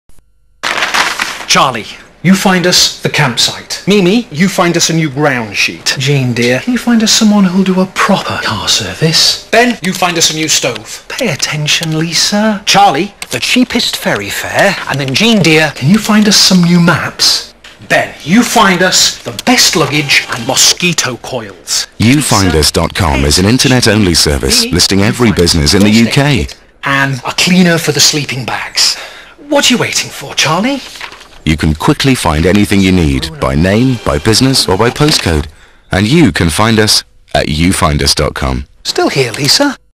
UFindUs TV advert - 2
The "Holiday" and how UFindUs helped the family get organised.